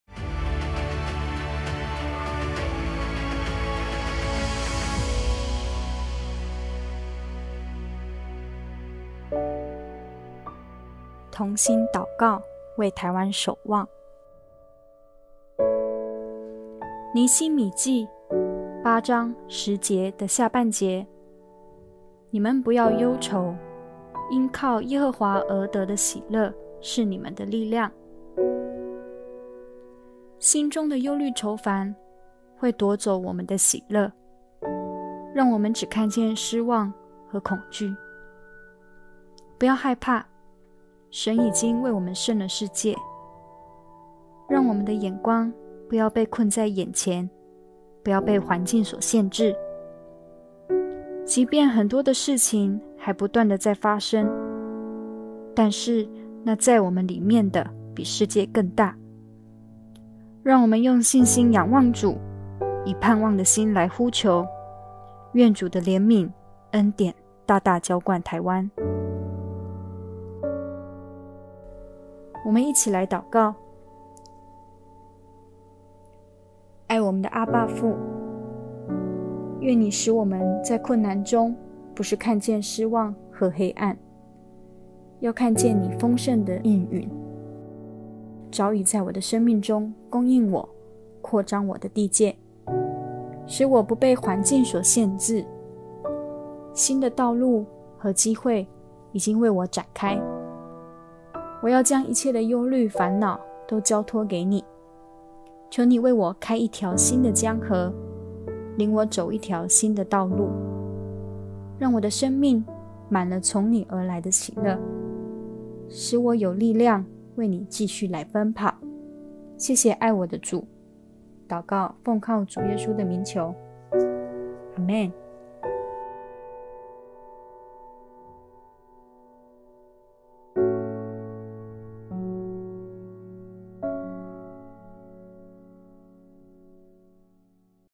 【同心禱告】2021/05/22
同心禱告  守望台灣

『你們不要憂愁，因靠耶和華而得的喜樂是你們的力量。』 --尼希米記 8:10b

心中的憂慮愁煩會奪走我們的喜樂，讓我們只看見失望和恐懼
不要害怕，神已為我們勝了世界
讓我們的眼光不要被困在眼前，不要被環境所限制
即便很多的事情還不斷地在發生，但那在我們裡面的比世界更大
讓我們用信心仰望主，以盼望的心來呼求
願主的恩典憐憫大大澆灌

讓我們在困難中不是看見失望和黑暗，我要看見主你的豐盛，早已在我的生命中供應我，擴張我的地界，不要被環境所限制，新的道路和機會已經為我展開，我將一切的憂慮煩惱都交託給祢，你為我開一條新的江河，領我走一條新的道路，我的生命滿了從你而來的喜樂，使我的有力量為祢繼續來奔跑

每天晚上10點與您一起來禱告

為台灣禱告運動。為台灣加油守望

患難時刻論壇不休息，與我們並肩同行！

如果你喜歡我們的影片，現在就立刻行動，按訂閱支持我們哦！